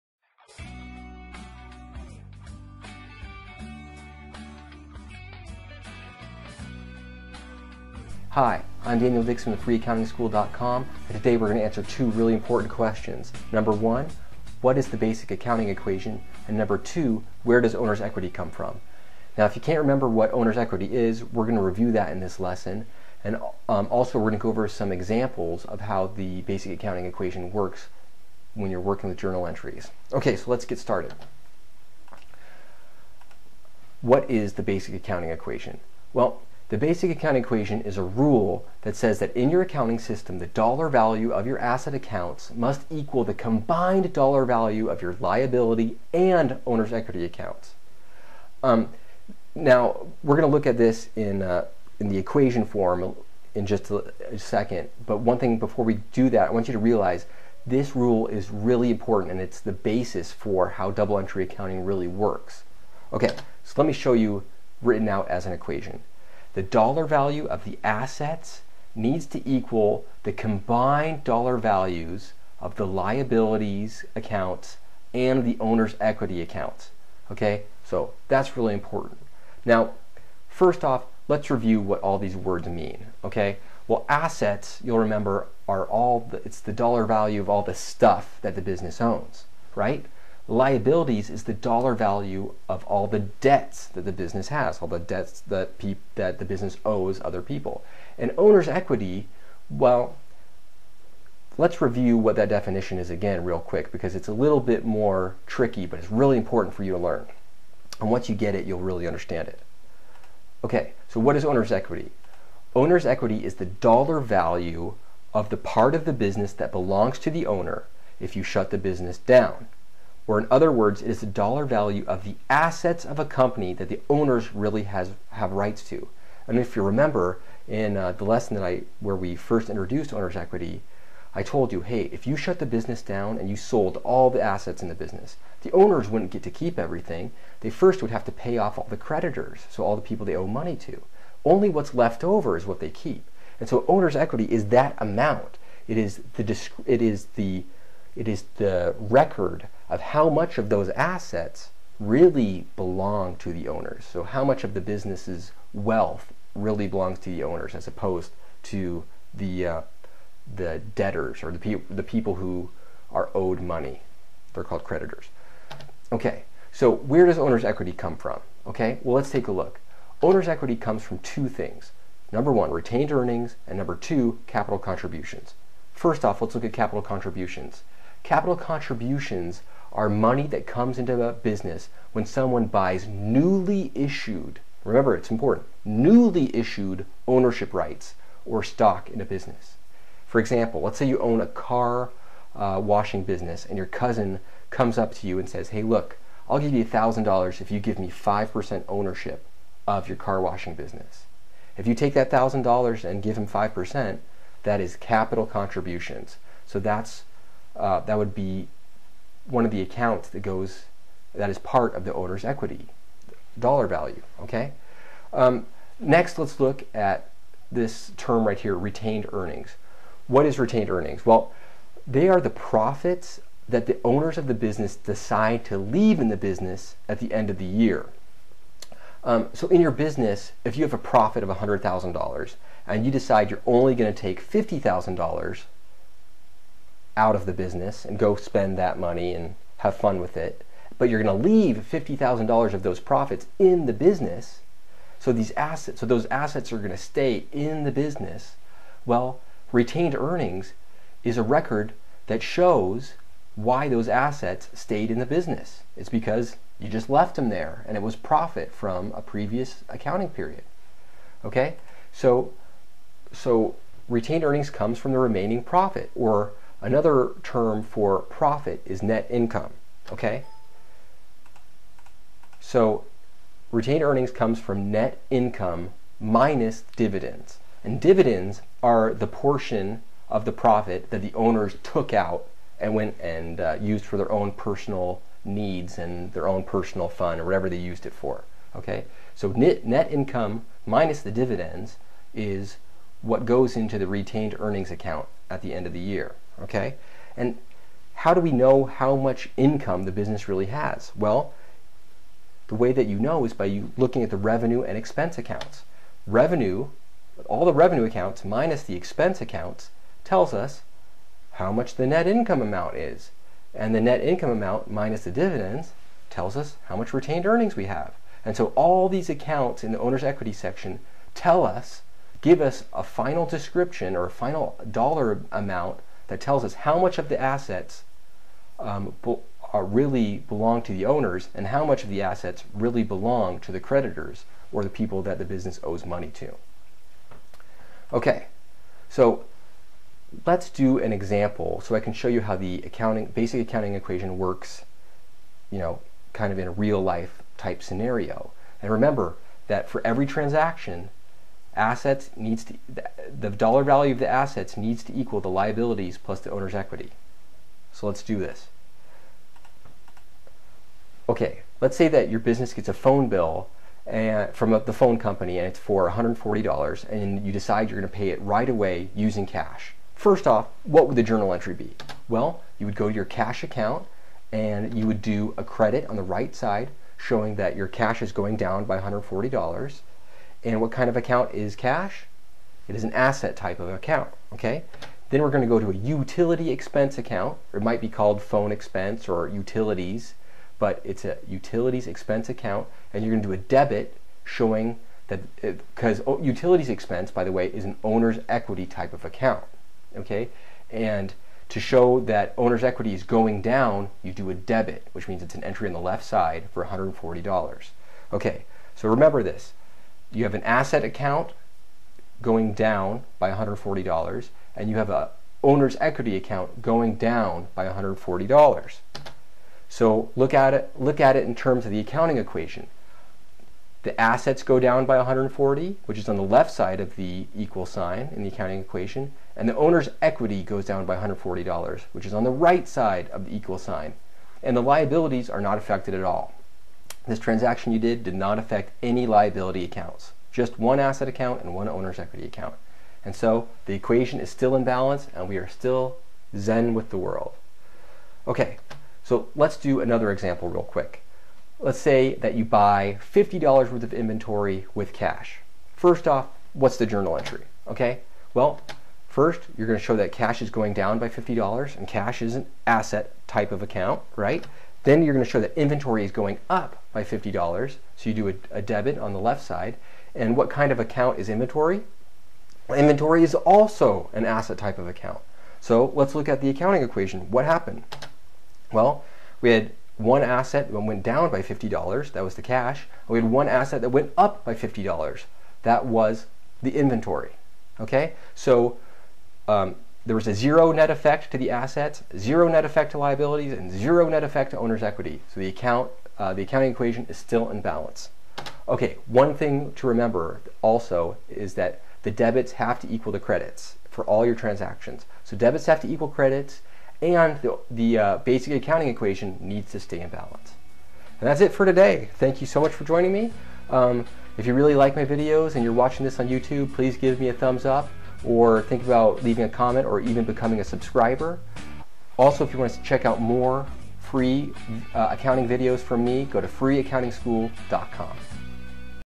Accounting Basics Lesson 6: The Accounting Equation and Where Does Owner's Equity Come From
In this tutorial, accountant Daniel Dickson answers the following questions: What is the Basic Accounting Equation?  Where Does Owner's Equity Come From?